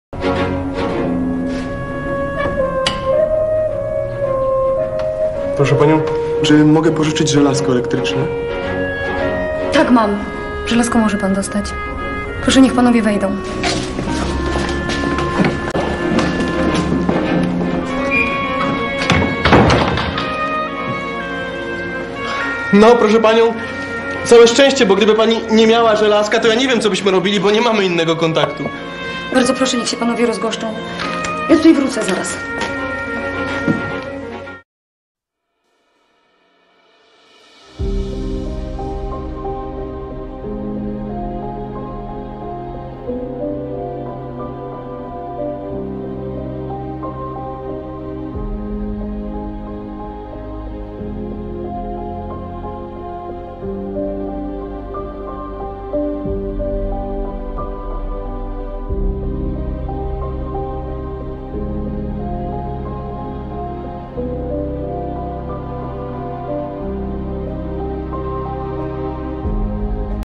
Reface (Vincent van Gogh) Maciej Skuratowicz - electric iron (key word)@adamplotagonart
Tezeusz puka do drzwi i mówi hasło czy ma Pani żelazko (słowo klucz) - Pani ma żelazko czyli wpuszcza Tezeusza na senans spirytystyczny